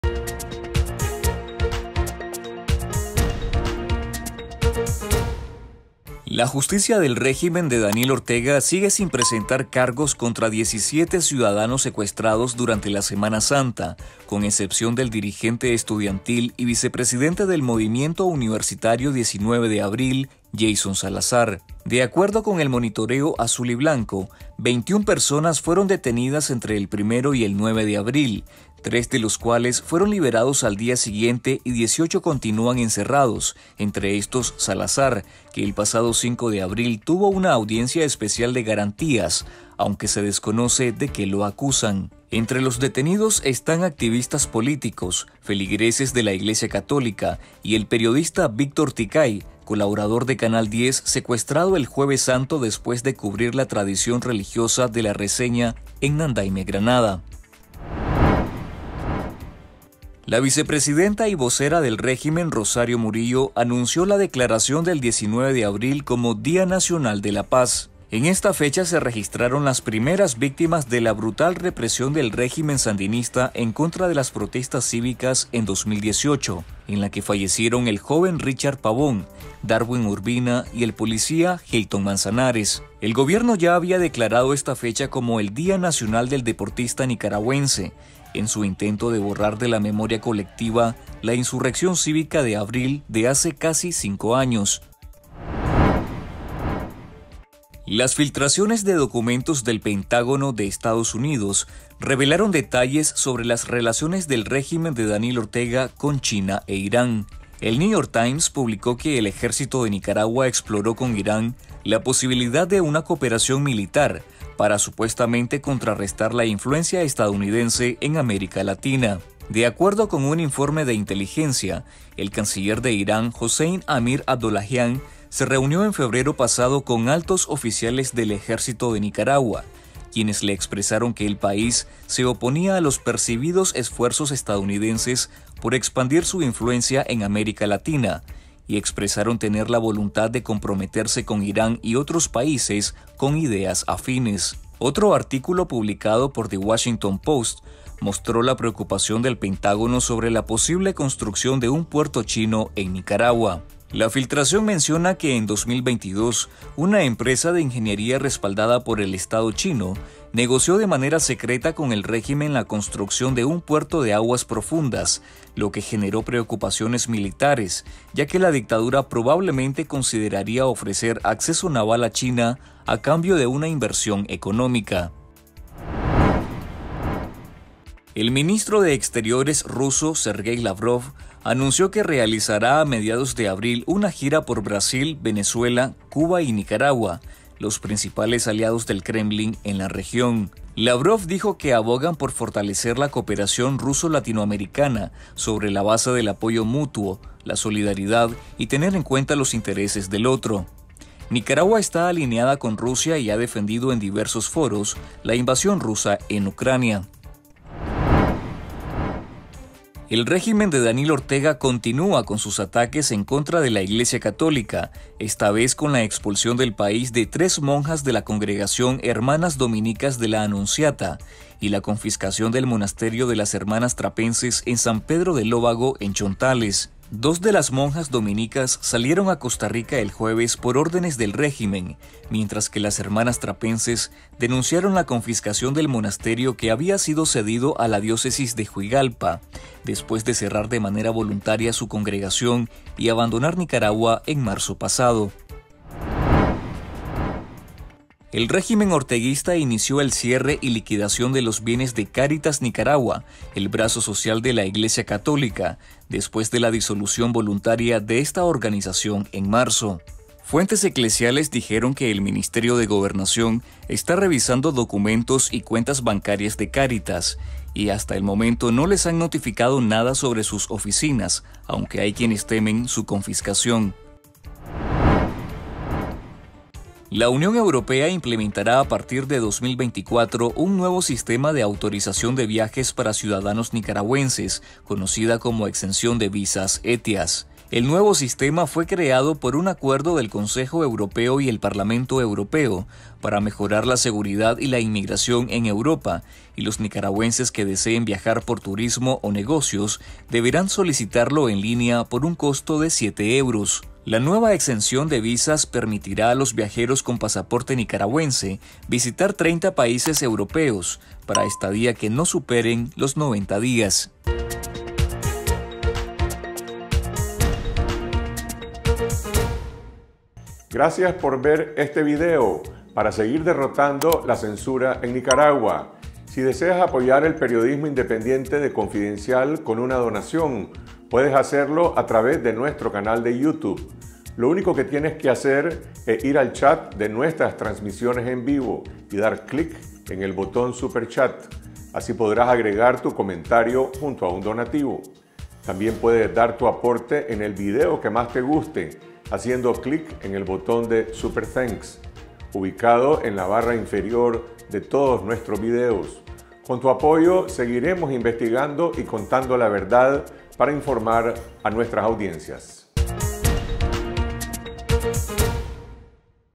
17 detenidos en Semana Santa siguen encerrados "sin ninguna acusación" del régimen
La justicia del régimen de Daniel Ortega sigue sin presentar cargos contra 17  ciudadanos secuestrados durante la Semana Santa, con excepción del dirigente estudiantil y vicepresidente del Movimiento Universitario 19 de Abril, Jasson Salazar.
De acuerdo con el Monitoreo Azul y Blanco, 21 personas fueron detenidas entre el 1 y 9 de abril, tres de los cuales fueron liberados al día siguiente y 18 continúan encerrados, entre estos Salazar, que el pasado 5 de abril tuvo su audiencia especial de garantías, aunque se desconoce de qué lo acusan.
Entre los detenidos están activistas políticos, feligreses de la Iglesia católica y el periodista Víctor Ticay, colaborador del Canal 10, secuestrado el Jueves Santo después de cubrir la tradición religiosa de La Reseña en Nandaime, Granada.

Estas son otras noticias de nuestro Informe Semanal:

- Murillo declara Día de la Paz el 19 de abril en que empezó la masacre.
- Filtraciones del Pentágono sobre relaciones de China e Irán con Ortega.
- Canciller ruso visitará Nicaragua, Brasil, Venezuela y Cuba.
- Siguen ataques a la iglesia católica con expulsión de monjas y confiscación de monasterio.
- Régimen inició cierre y liquidación de Cáritas Nicaragua.
- Unión Europea crea nuevo sistema de autorización de viajes para nicaragüenses.

Los nicaragüenses merecen información confiable yde calidad. Apoyá nuestra misión. Unite a nuestro Programa de Membresía 📺